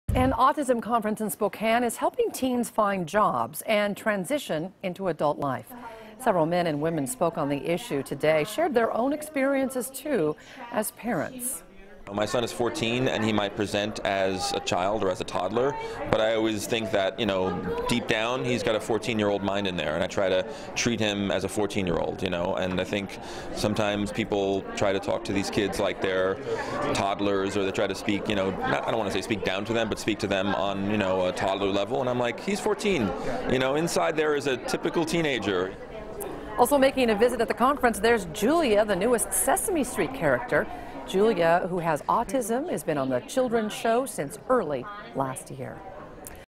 Spokane autism conference features special guest from Sesame
Hotel Red Lion hosted an autism conference all day in Spokane.